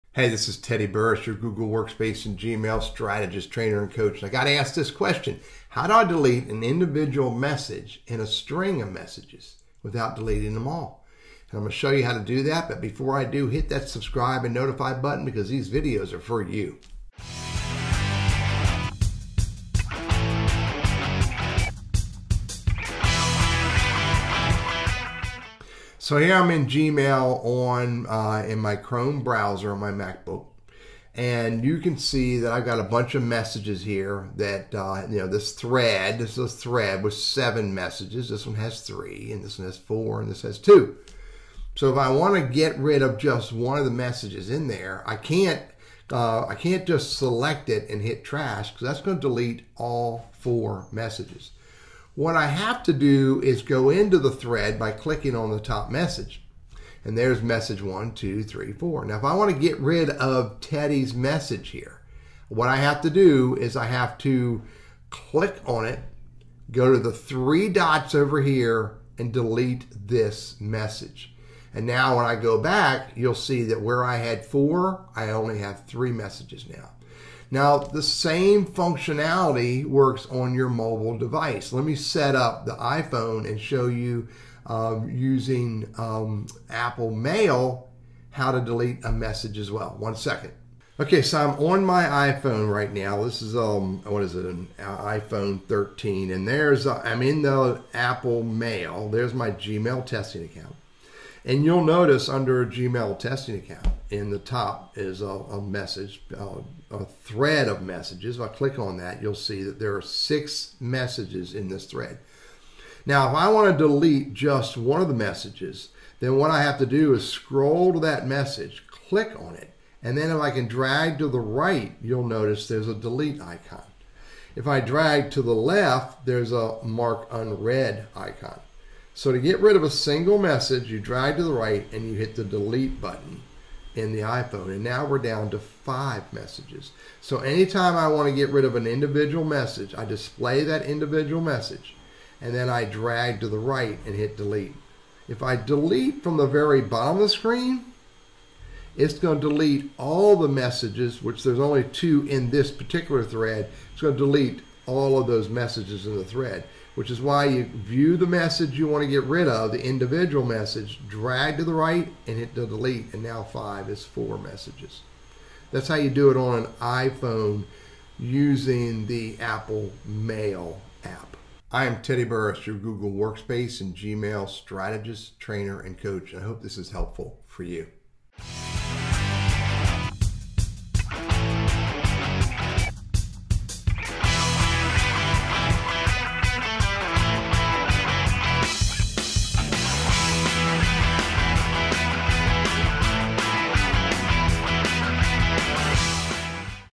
(No longer works) - Deleting single message in a Thread of Emails in Google Workspace or Gmail?
The steps in the Gmail App on a smartphone no longer work. Updated 10/16/25

I demonstrate how to delete a single message in a message thread on an iPhone or in a Google Web Browser using Gmail.

Updated thumbnail - 06/06/24